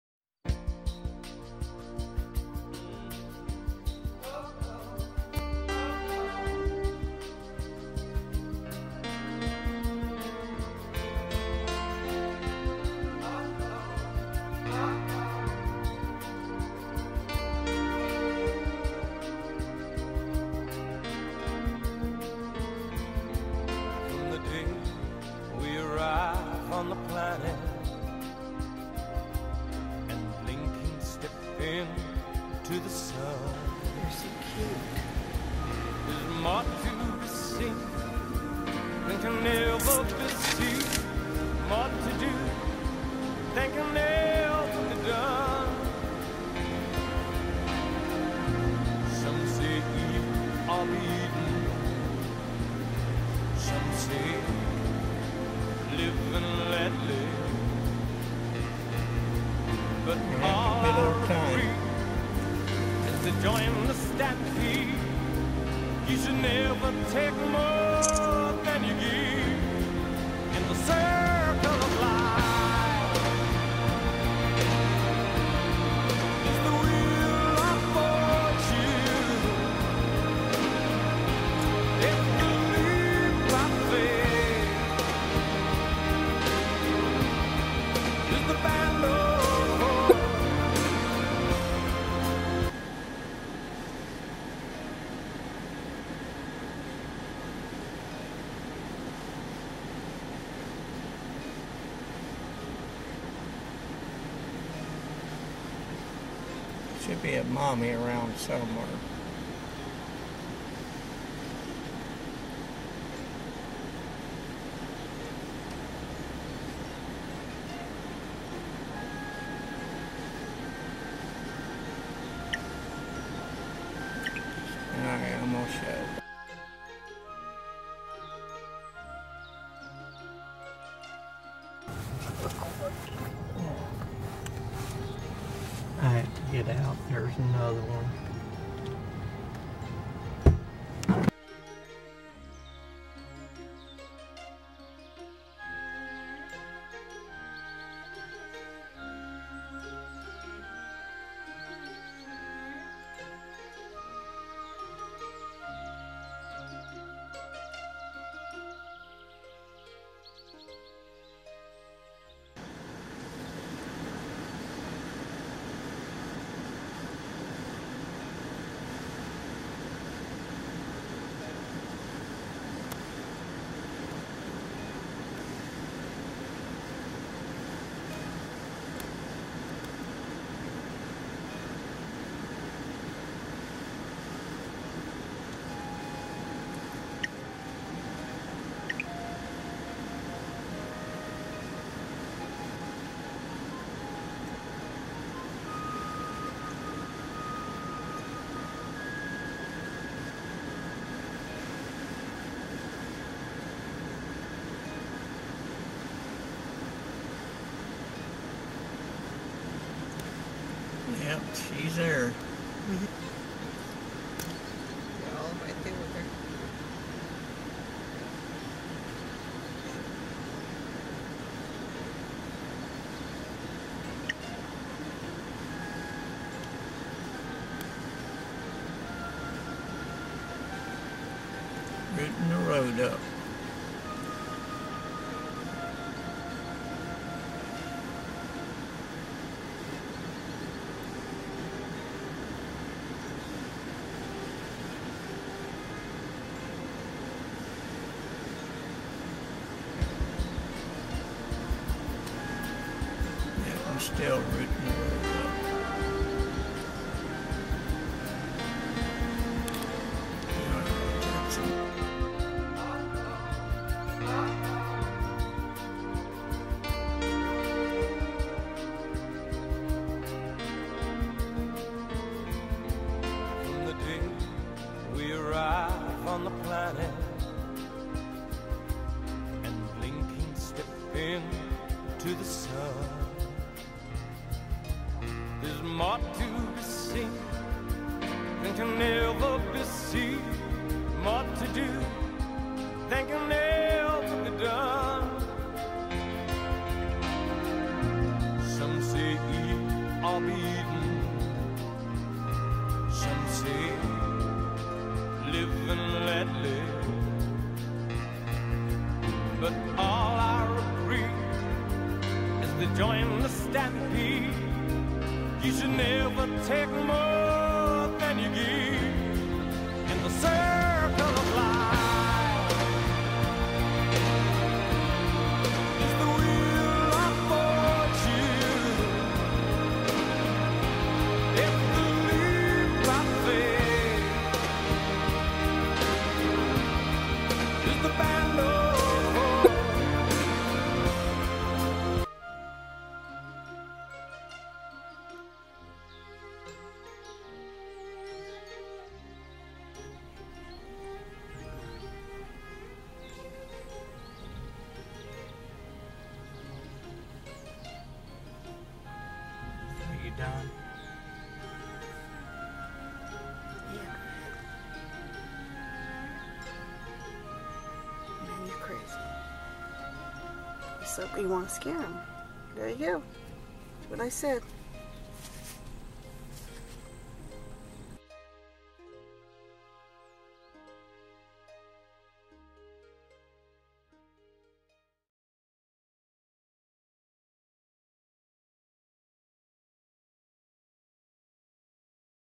Crows Nest Perry Florida wild hog videos
these videos have been combined to make this one video. Several days are involved but all in the afternoon and all in May of 2015.